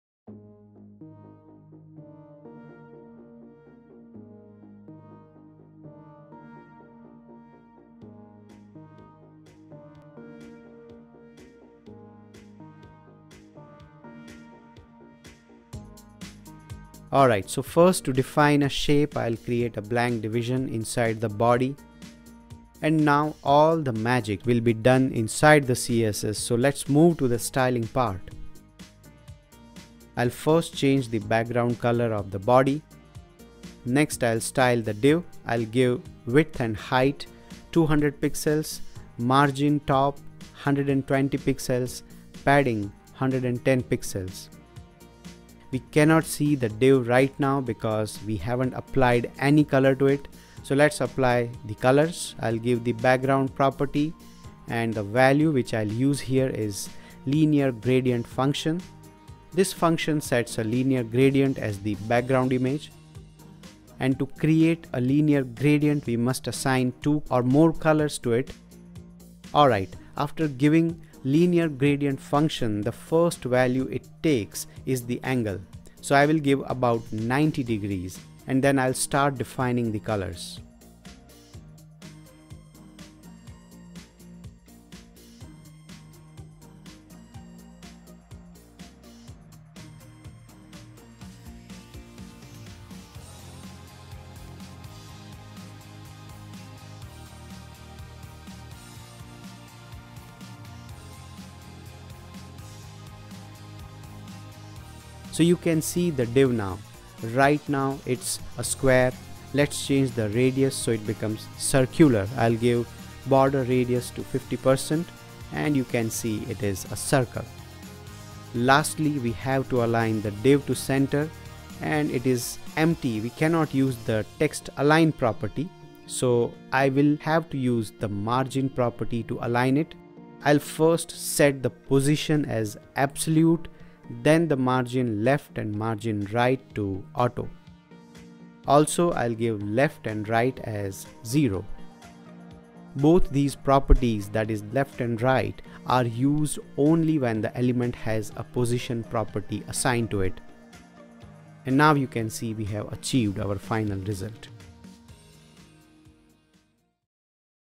Rainbow Colored Circle with CSS HSL() | HSL() Color Values | Coding Exercise | AnaghTech
We have seen earlier how to create basic & advanced shapes with Pure CSS & now in this video, we’ll do a CSS coding exercise.
We’ll create a circle and then assign rainbow colors to it using hsl() color values.